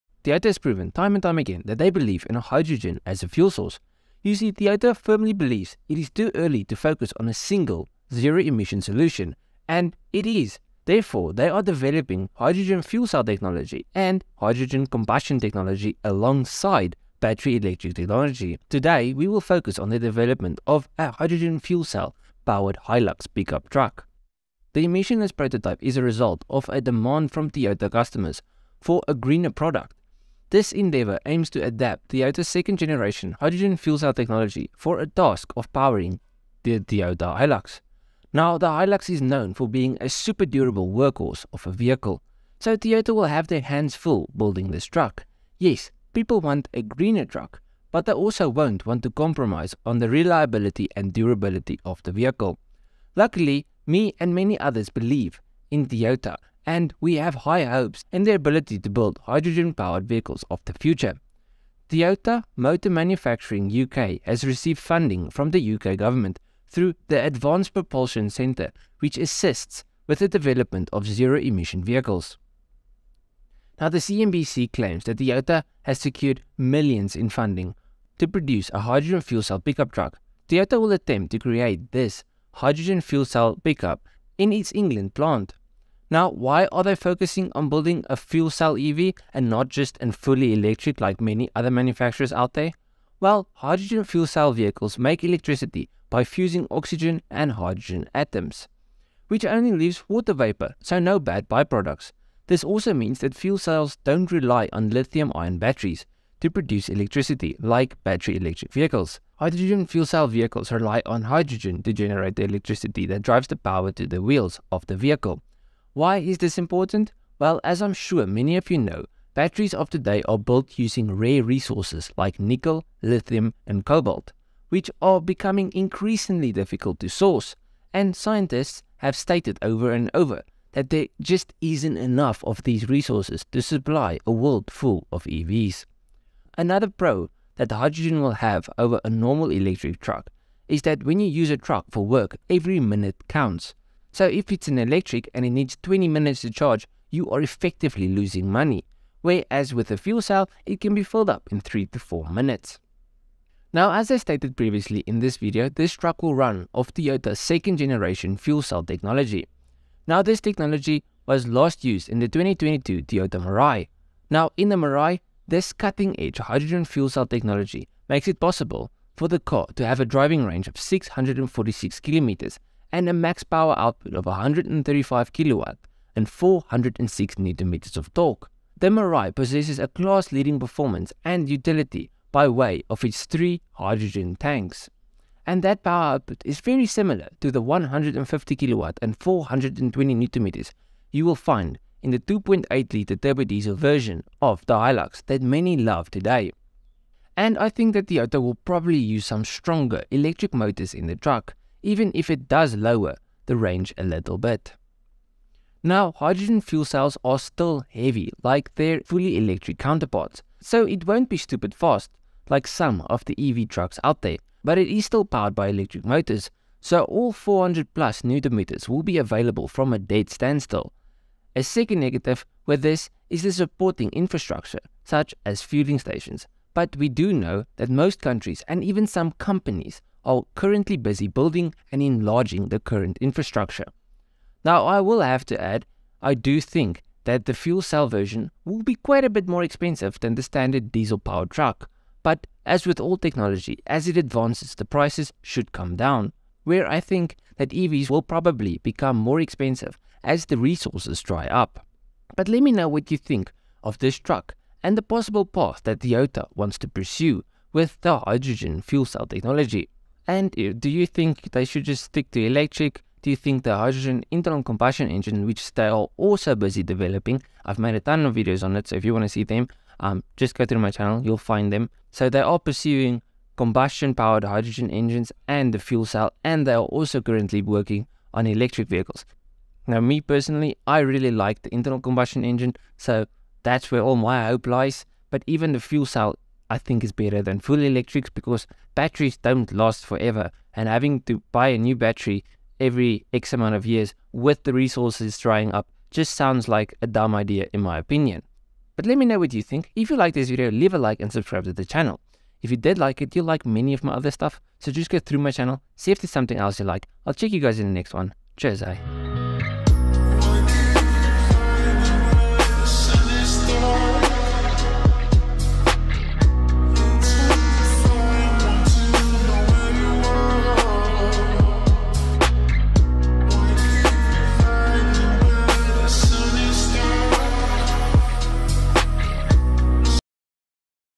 The Development of the Hydrogen Fuel cell powered Hilux has started
Toyota has proven that they believe in Hydrogen as a fuel source, you see Toyota firmly believes it is too early to focus on one single zero-emission solution and is, therefore, they are developing hydrogen fuel cell technology and hydrogen combustion technology alongside battery electric technology. Today we will focus on their development of a hydrogen fuel cell powered hilux pick up.

The emission-less prototype is the result of a demand from Toyota customers for a greener product. This endeavour aims to adapt Toyota’s second-generation hydrogen fuel cell technology for the task of powering a Toyota Hilux. Now the Hilux is known for being a super durable workhorse of a vehicle so Toyota will have their hand full building this truck. Yes people want a greener truck, but they also wont want to compromise on reliability and durability. Luckily me and many other believe in Toyota and we have high hopes in their ability to build Hydrogen vehicles of the future. Toyota Motor Manufacturing UK has received funding from the UK government through the Advanced Propulsion Centre which assists with the development of zero-emission vehicles.

Why is this important? Well as im sure many of you know the batteries of today are build using rare resources like, nickel, litium and cobalt which are becoming increasingly difficult to source. And scientists have stated over and over that there isn’t enough of these resources to supply a world full of EV’s.

Another pro that hydrogen will have over a normal electric truck, when you use your truck for work every minute counts so if its electric and it needs 20 mins to charge you are effectively losing money, where as with a fuel cell it can be filled up in 3-4 minutes.